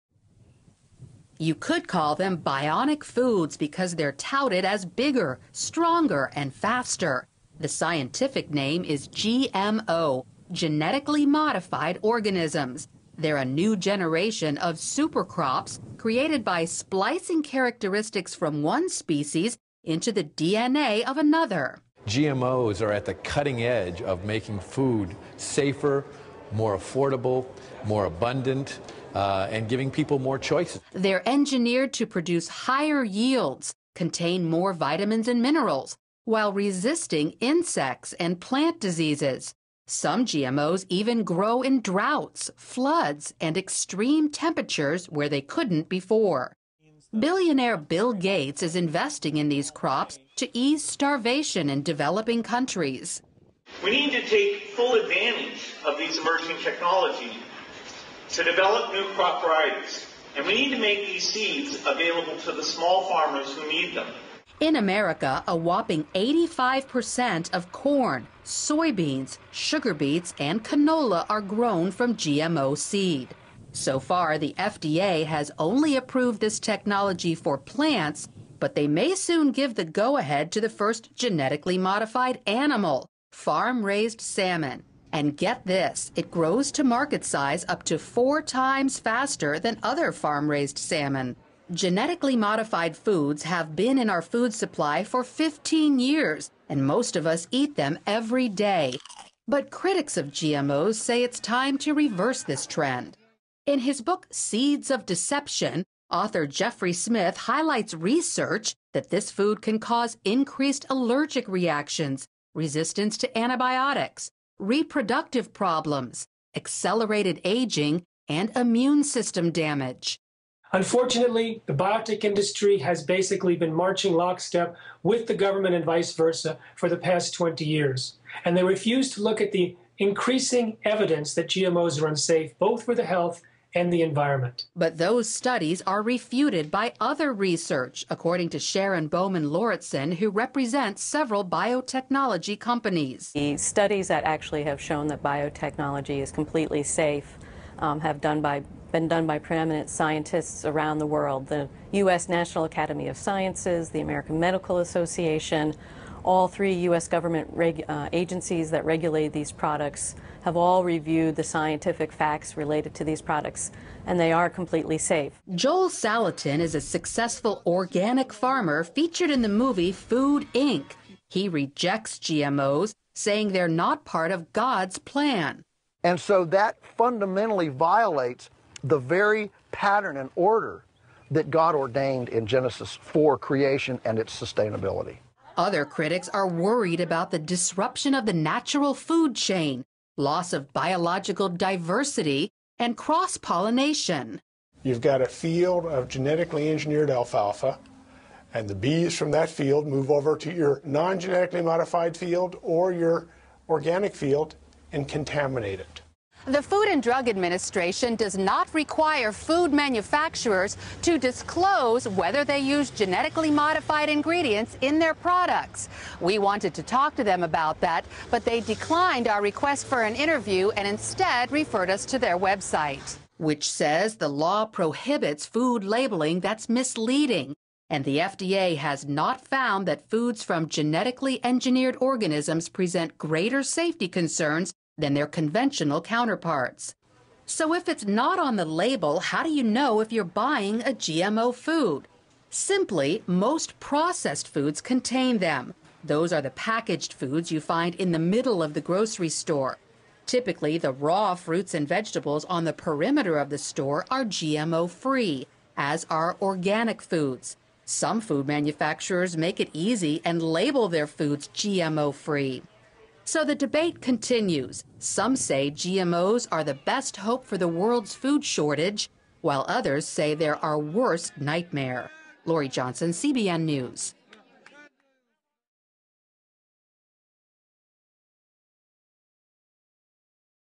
'Bionic' Fight: Genetically Modified Foods Safe? - CBN.com
Genetically modified foods have been in our food supply for 15 years and most of us eat them every day. But critics say it's time to reverse this trend... The Christian Broadcasting Network CBN